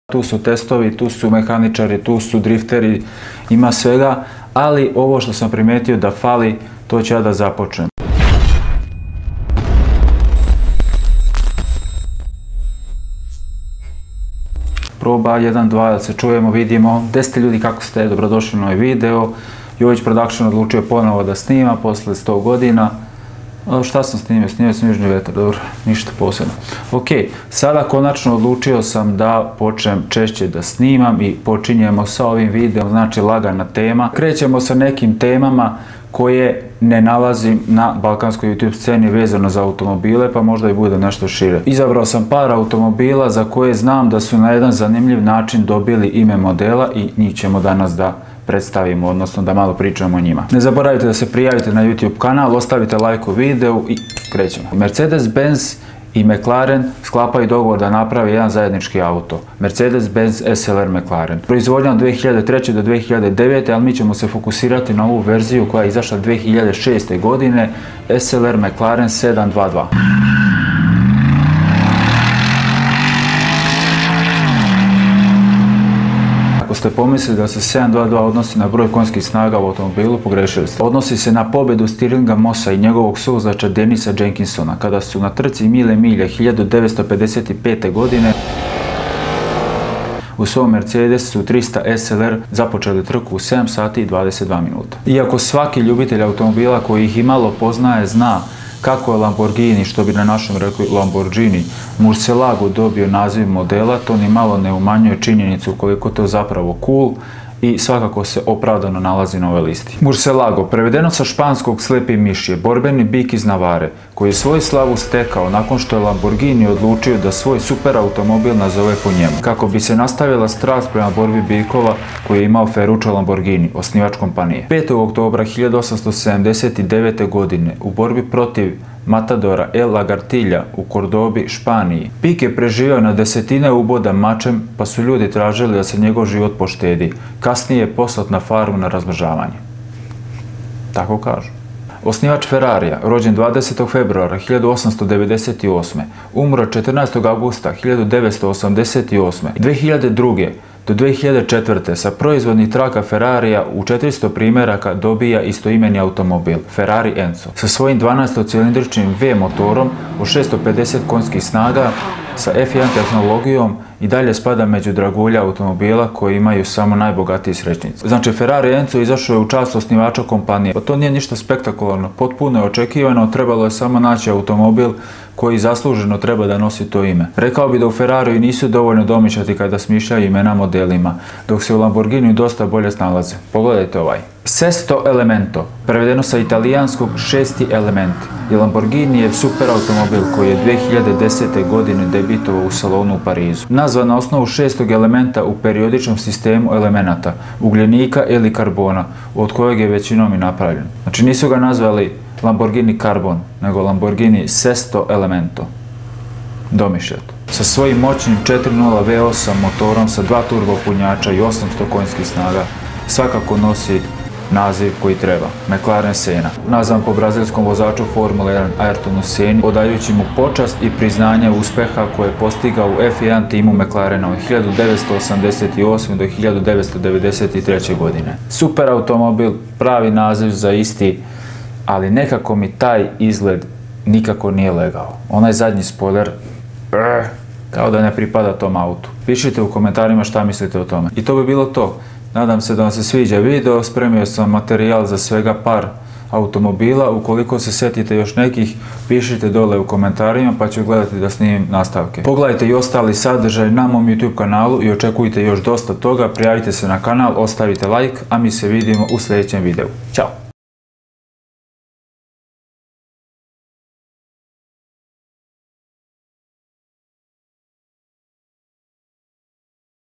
KAKO SU NAZVANI NEKI AUTOMOBILI
Ispričao sam kako su neki od automobila poput SLR McLarena 722, Lamborghini Sesto Elemento-a i ostalih dobili ime.
Uživajte u malo edukativno zabavnom sadržaju :)